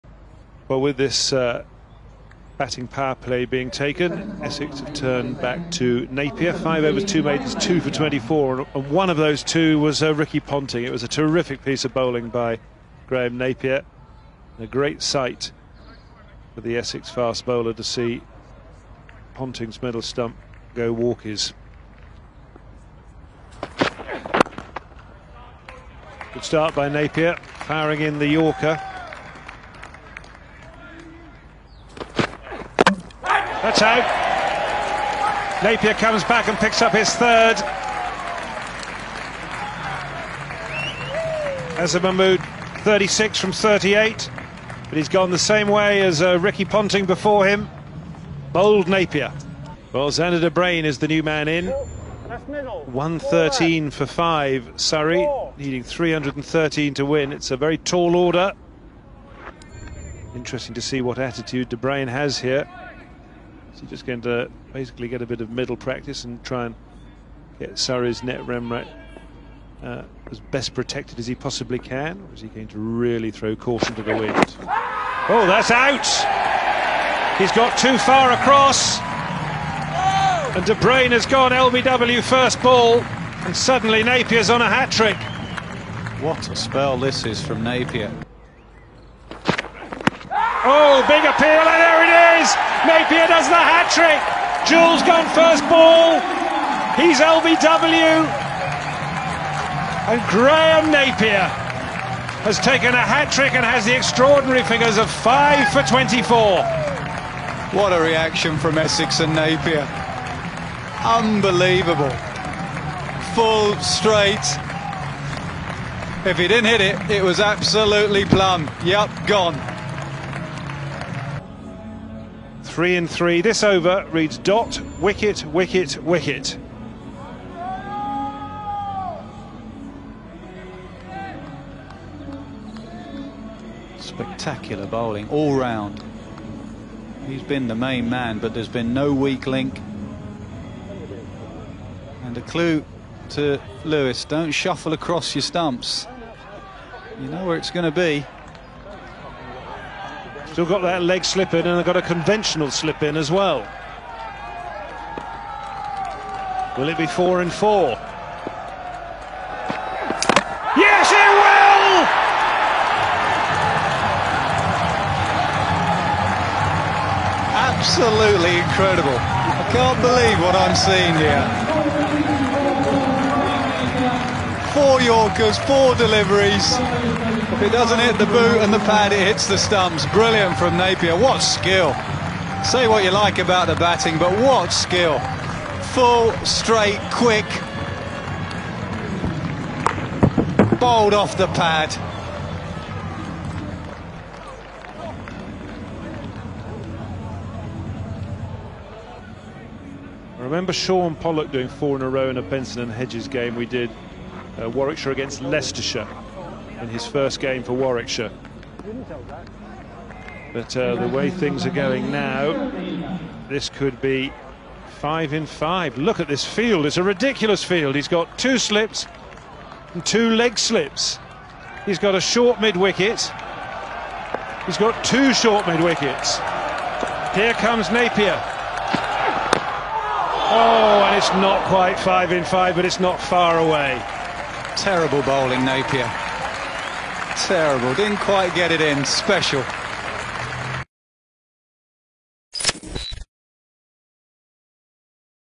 Graham Napier takes 4 wickets in 4 balls against Surrey (2013)
Watch one of the most memorable moments from Club legend, Graham Napier who took 4 wickets in 4 balls against Surrey in the Yorkshire Bank 40 match at Chelmsford in 2013.